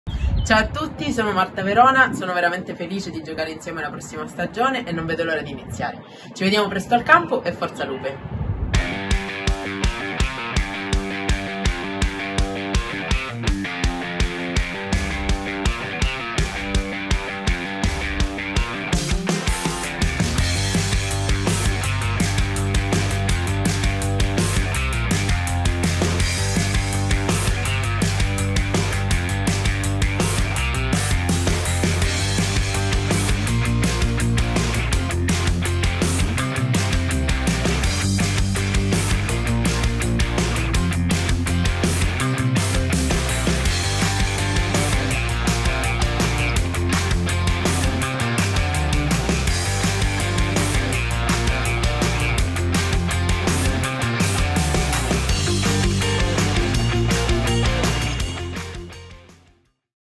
Welcome to Fila San Martino 2022/23: Marta Verona
Il saluto di Marta Verona, ala classe 1994, nei giocatrice del Fila San Martino di Lupari per la stagione 2022/23, e alcuni highlights della sua ultima stagione con la maglia di Patti.